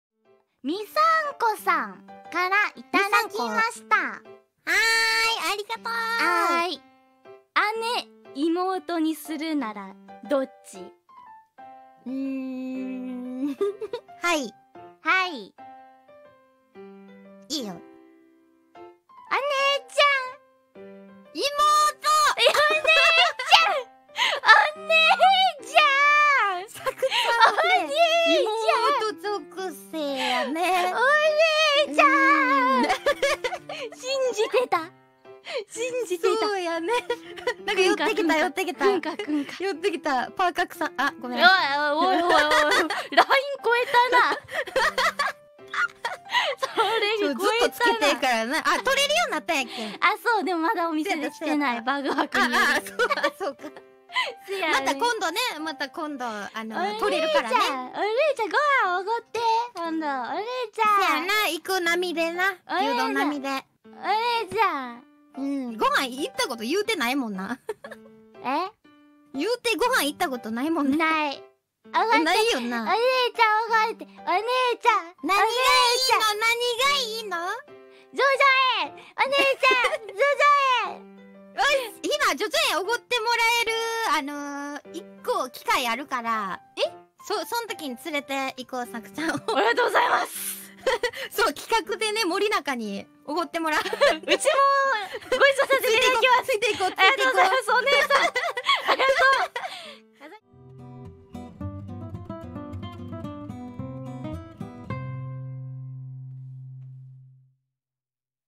[Nijisanji/Eng sub]Himawari and Sasaki, which one is little sister and older sister?
they had this little sis or big sis is because they have same artist(their mama)